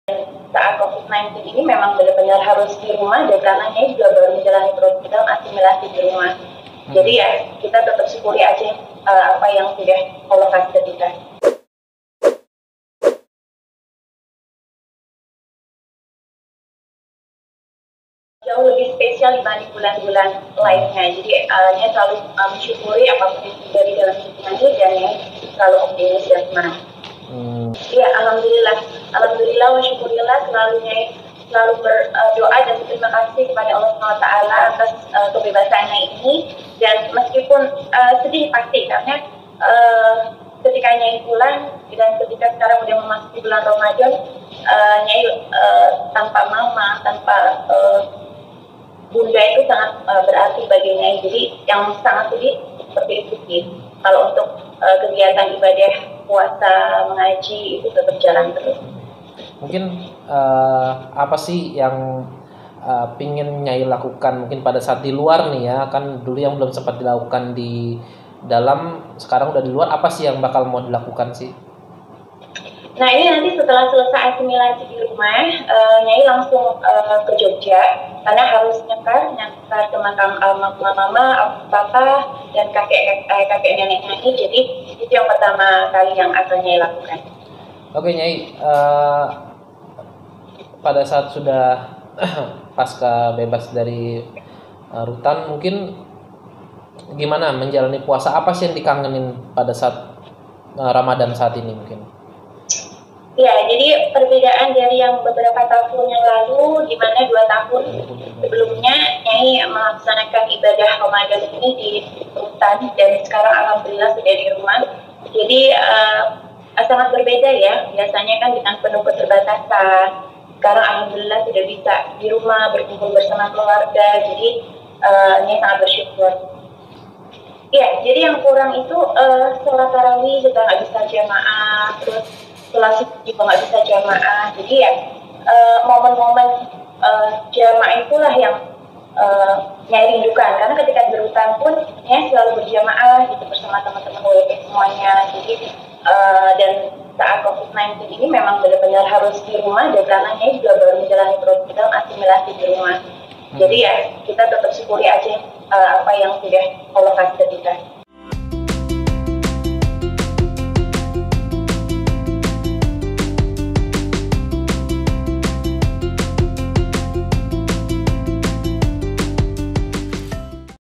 Roro Fitria Berusaha Jalani Kehidupan Saat Ini dengan Penuh Rasa Syukur
Roro Fitria memaknai kehidupannya saat ini sangat berbeda, sebelum ia mendapatkan cobaan, mendekam di balik jeruji besi. Kepada Beepdo.com, Minggu (26/4) melalui sambungan video call, Roro tegaskan, saat ini menjalani kehidupannya dengan penuh rasa syukur. Bagaimana penuturan selengkapnya? (Gede/Beepdo.com)